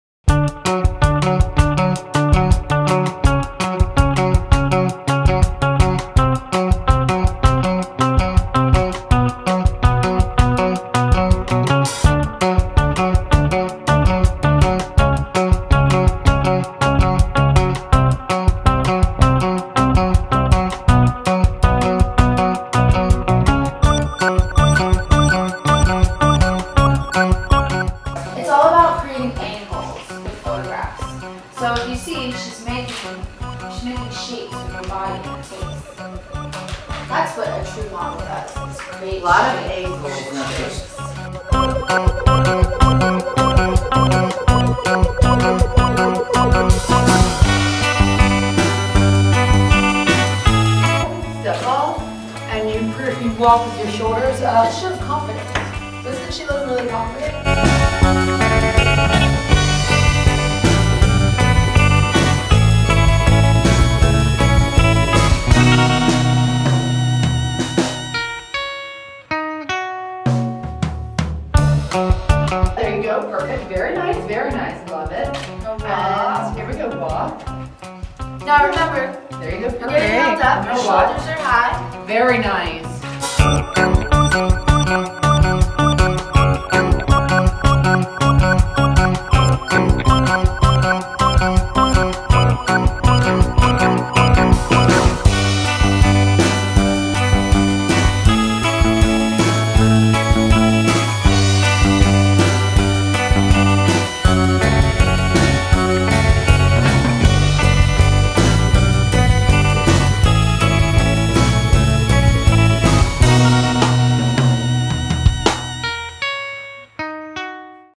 Supermodels Unlimited Magazine :: Ayana's Top Model Weekend with Brittany Brower & Lisa D'Amato
SU VIP, Ayana learns from the best at our recent shoot in Park City, Utah. America's Next Top Model All-Stars, Lisa D'Amato and Brittany Brower teach tips & tricks to being a world class professional model.